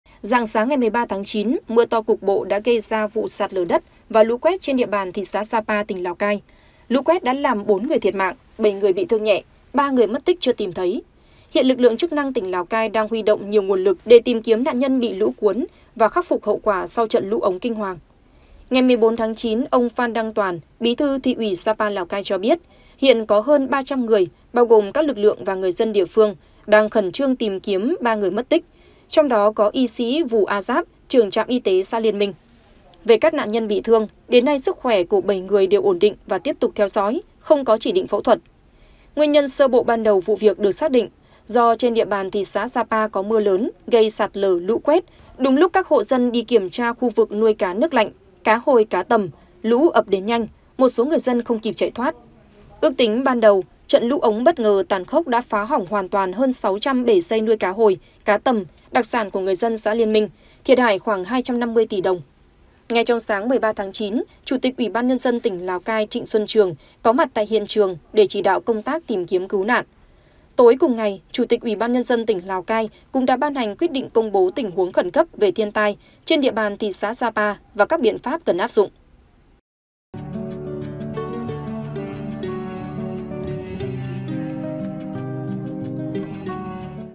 Huy Động Hàng Trăm Người Tìm Kiếm 3 Nạn Nhân Mất Tích Do Lũ Dâng Kinh Hoàng Ở Sa Pa, Lào Cai | SKĐS
SKĐS | Rạng sáng ngày 13/9, mưa to cục bộ đã gây ra vụ sạt lở đất và lũ quét trên địa bàn Thị xã Sa Pa, tỉnh Lào Cai. Lũ quét đã làm 4 người thiệt mạng, 7 người bị thương nhẹ, 3 người mất tích chưa tìm thấy. Hiện lực lượng chức năng tỉnh Lào Cai đang huy động nhiều nguồn lực để tìm kiếm nạn nhân bị lũ cuốn và khắc phục hậu quả sau trận lũ ống kinh hoàng.

Mời quý vị và các bạn xem thêm: Vụ Cháy Chung Cư Mini Ở Khương Hạ: Bé Trai Nằm Viện Một Mình Vỡ Òa Khi Đã Tìm Được Bố Mẹ